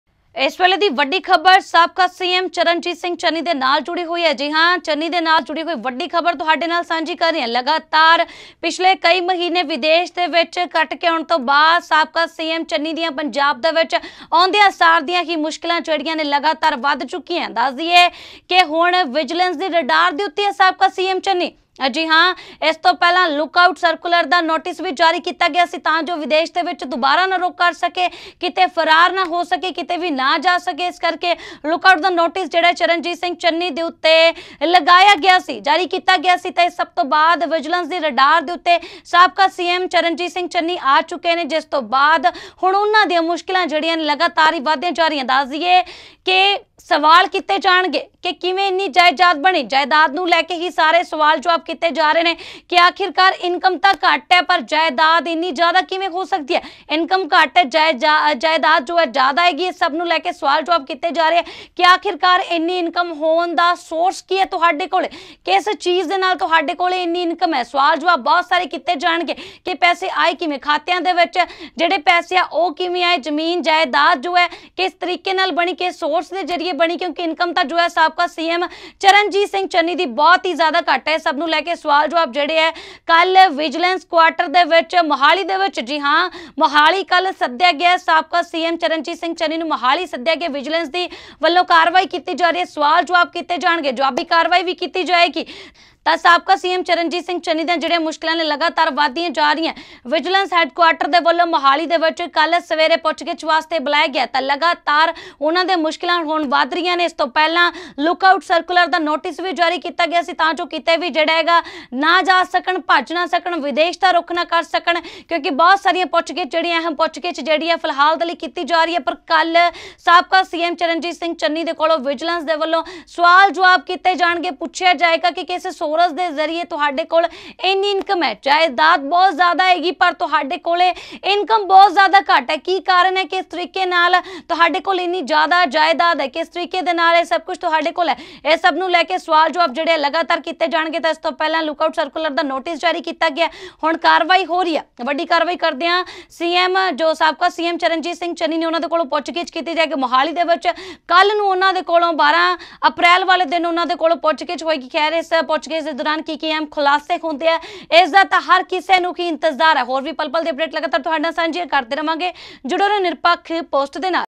ਵਿਜੀਲੈਂਸ ਦੀ ਰਡਾਰ ਤੇ ਚਰਨਜੀਤ ਸਿੰਘ ਚੰਨੀ ਚੰਨੀ ਖਿਲਾਫ ਪਹਿਲਾ ਤੋਂ ਜਾਰੀ ਹੈ ਲੂਕਆਊਟ ਨੋਟਿਸ
ਵਿਜੀਲੈਂਸ ਦੀ ਰਡਾਰ ਤੇ ਚਰਨਜੀਤ ਸਿੰਘ ਚੰਨੀ
ਚੰਨੀ ਖਿਲਾਫ ਪਹਿਲਾ ਤੋਂ ਜਾਰੀ ਹੈ ਲੂਕਆਊਟ ਨੋਟਿਸ

breakingnews,interviews,news,bignews
punjab,newsline,bignewsmedia,nirpakhpost